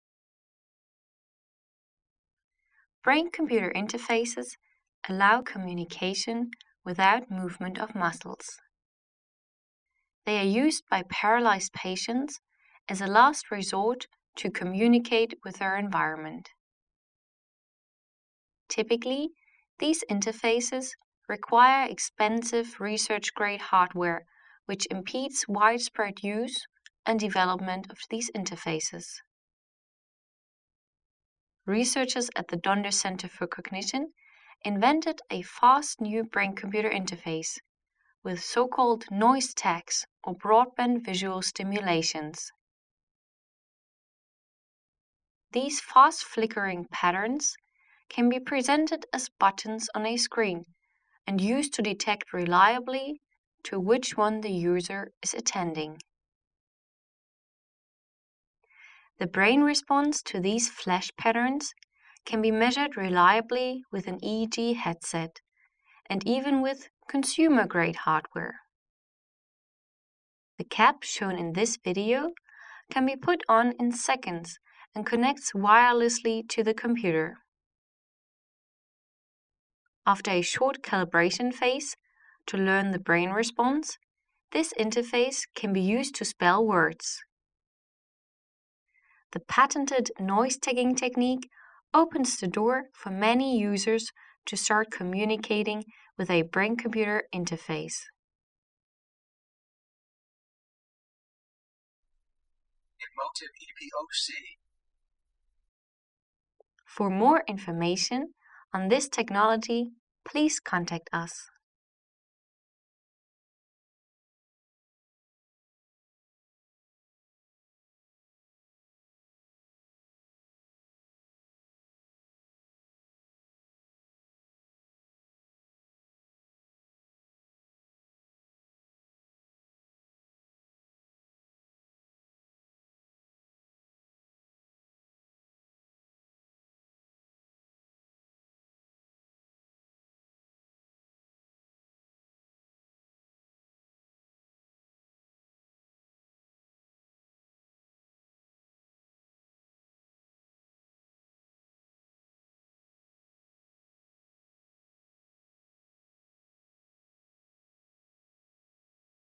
Brain Computer Interface with the Emotiv Epoc
A brain computer interface with consumer grade hardware is possible with the so-called noisetagging technique. This technique uses broad-band visual stimulations and reconvolution to find the brain response to individual flashes on a screen. It works so well that it does not need expensive EEG equipment.